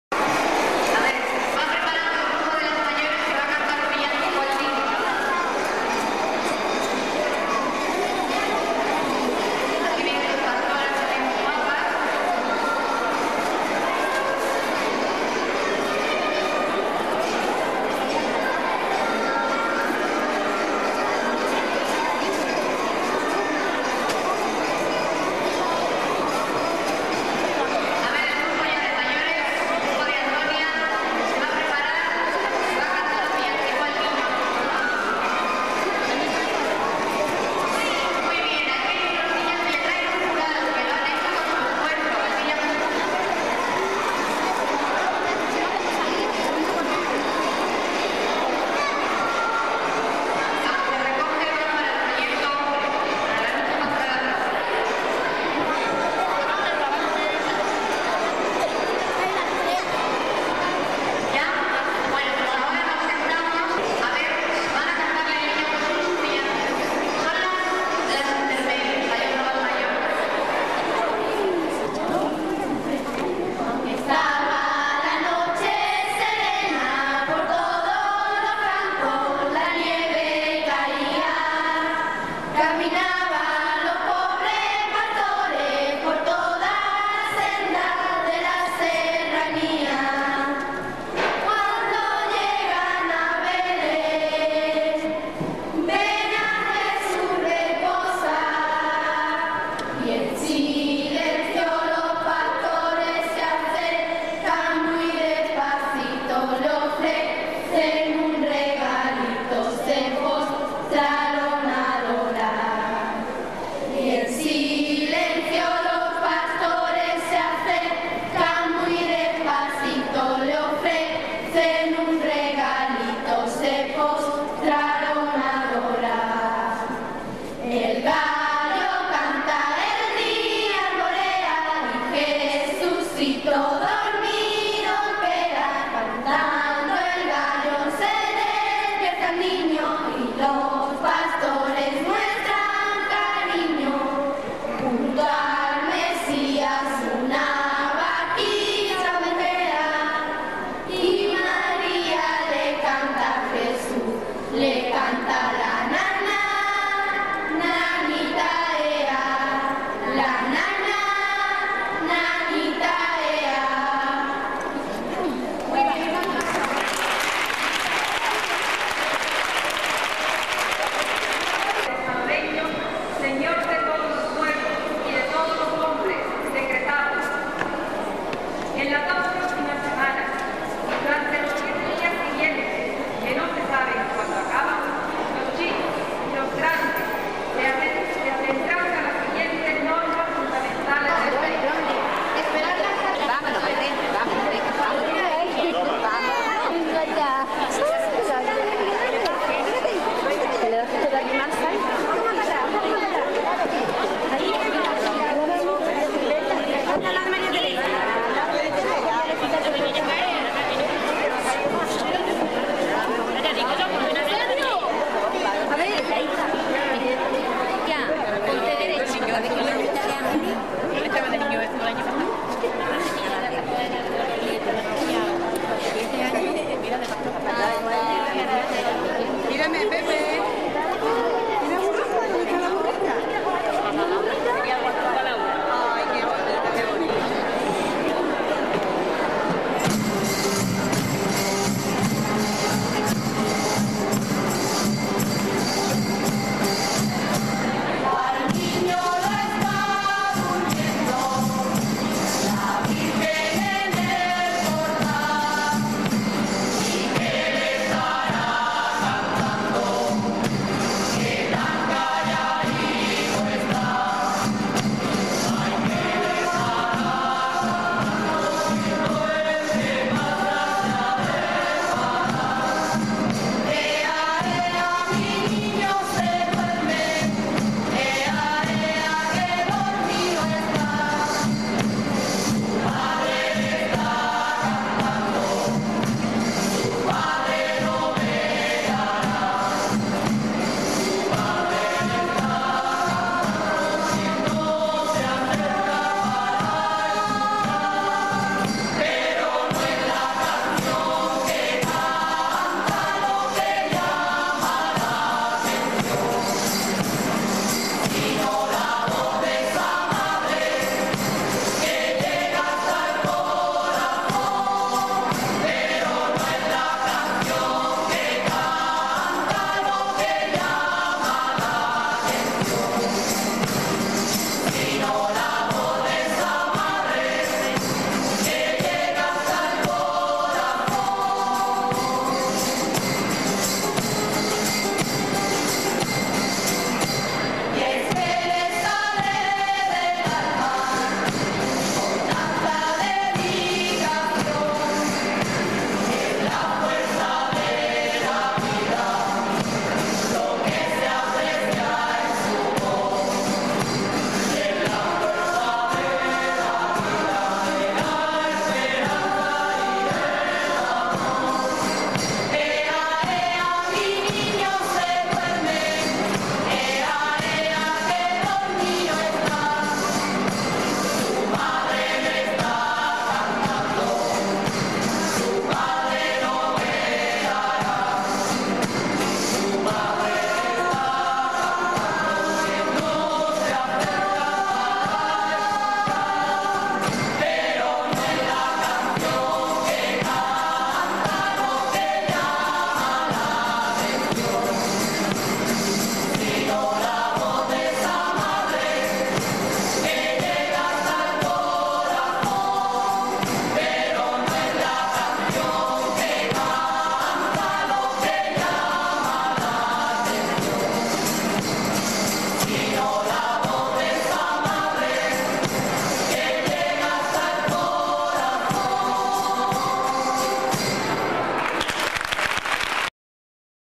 VILLANCICOS .-
Anoche un grupo de jovenes ivan cantando Villancicos por las calles del pueblo Ivan todos contentos .Se acerca la Noche buena la Navidad.